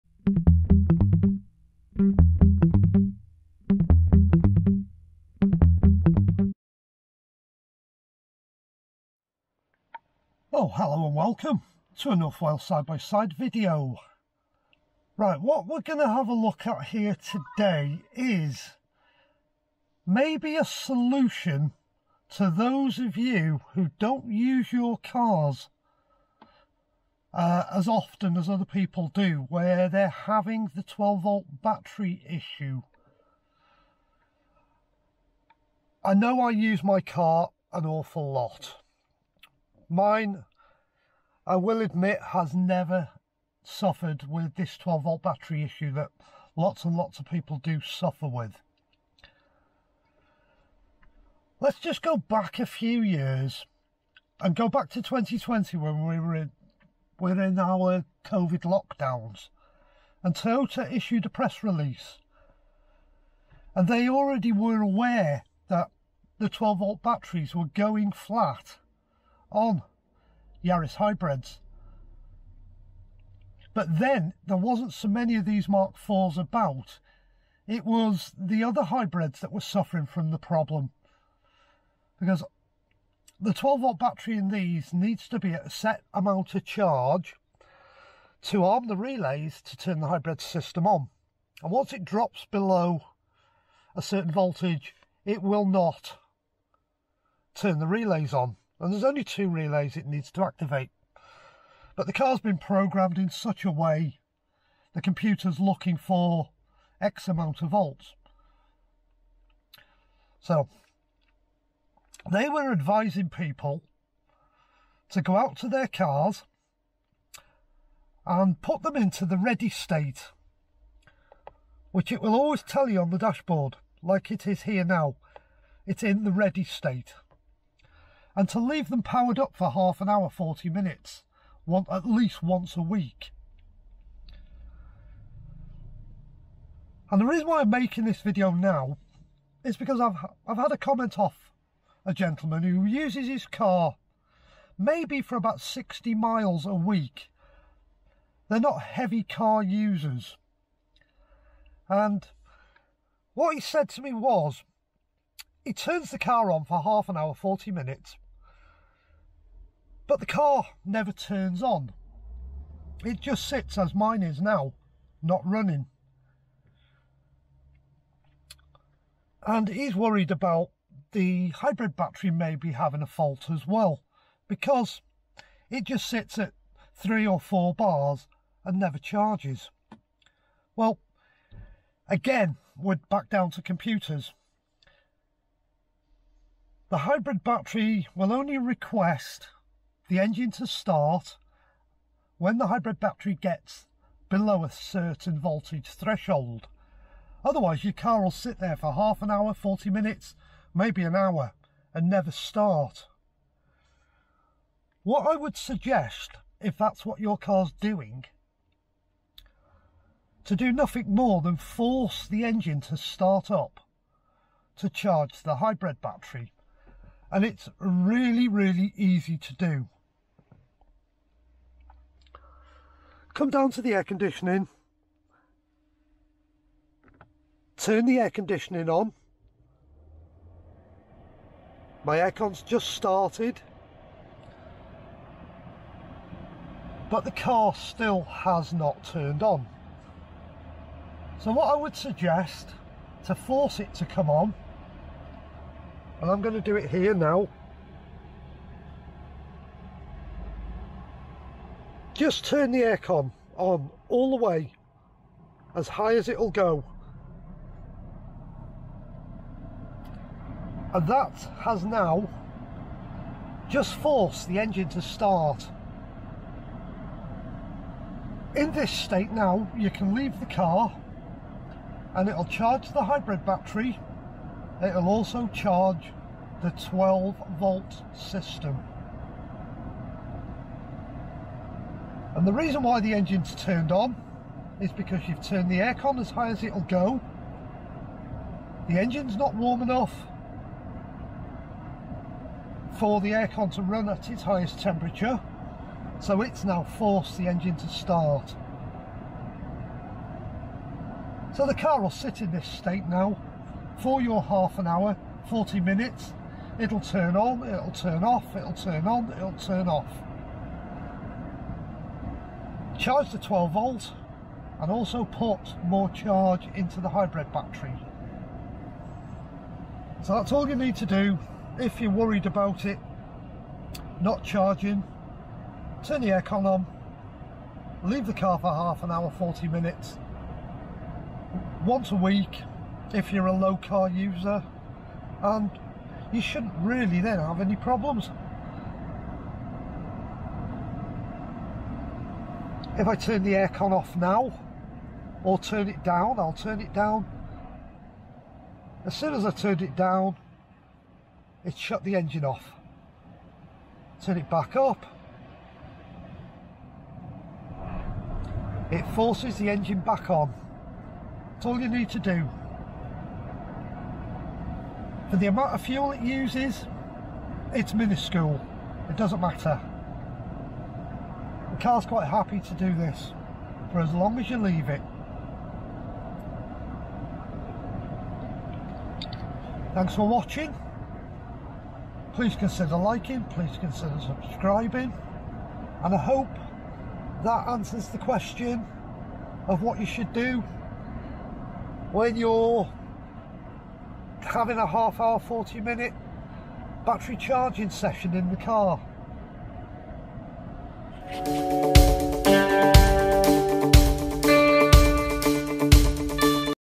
Tips & Tricks 47 How do you do a forced recharge of both 12 Volt and Hybrid battery
How do you do a forced recharge of both 12 Volt and Hybrid battery